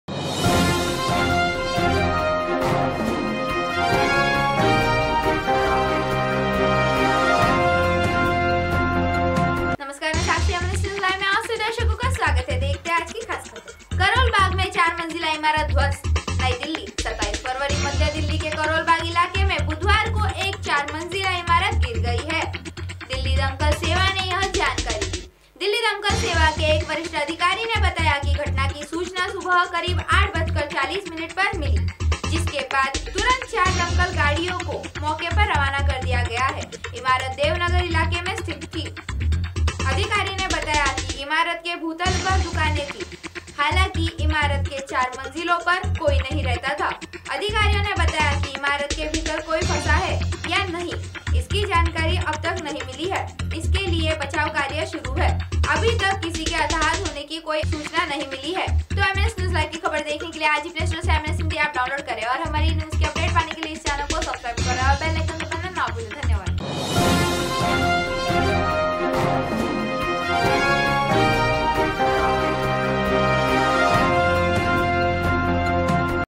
MNS NEWS LIVE IN HINDI करोलबाग में चार मंजिला ईमारत ध्वस्त
करोलबाग में चार मंजिला ईमारत ध्वस्त